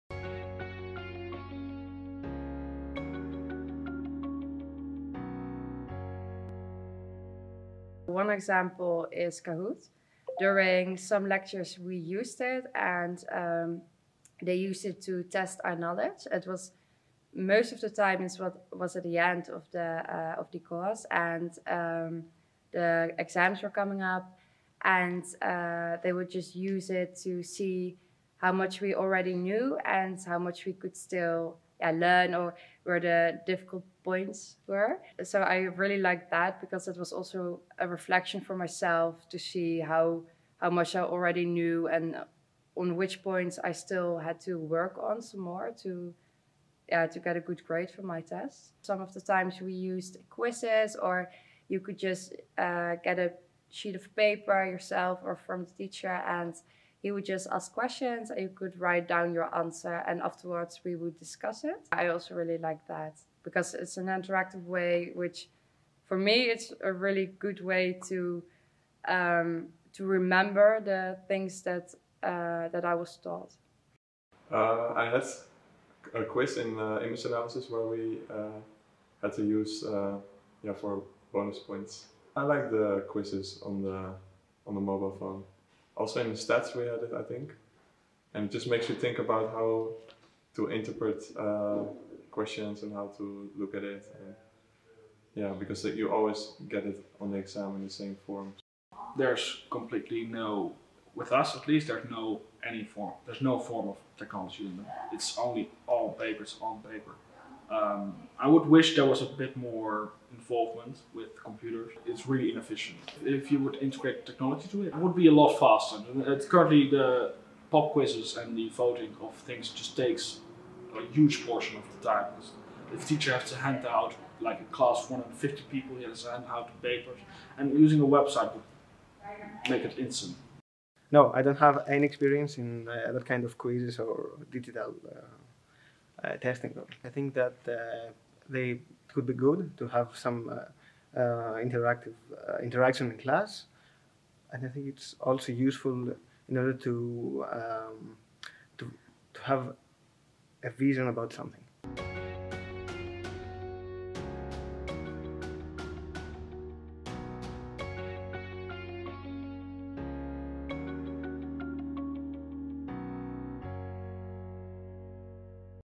Studenten ervaringen met digitaal testen en stemmen in de klas - Tilburg University
Studenten vertellen over hun ervaringen met digitaal testen en stemmen in de klas. In deze video vertellen ze over schillende methoden die gebruikt worden in de lessen, wat ze ervan vinden en waar ze graag meer van zouden willen zien.

Students talk about their experiences with digital quizzes and voting in class. In this video they talk about different methods used in the classroom, what they think of these methods and what they would like to see more of.